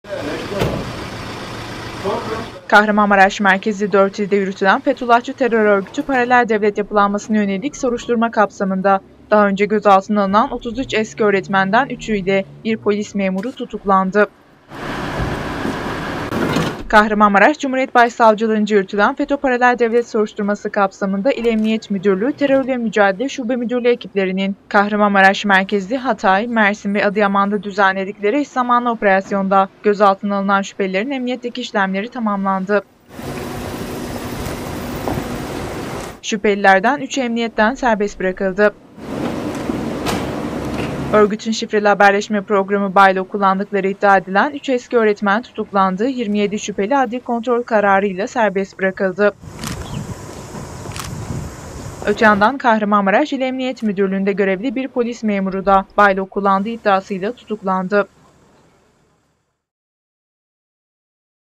"Bylock" Kullandığı Belirlenen 3 Eski Öğretmen İle 1 Polis Memuru Tutuklandı
Kahramanmaraş Merkezli 4 İlde Fetullahçı Terör Örgütü/Paralel Devlet Yapılanması'na (FETÖ/PDY) Yönelik Soruşturma Kapsamında Gözaltına Alınan 33 Eski Öğretmenden 3'ü İle 1 Polis Memuru Tutuklandı.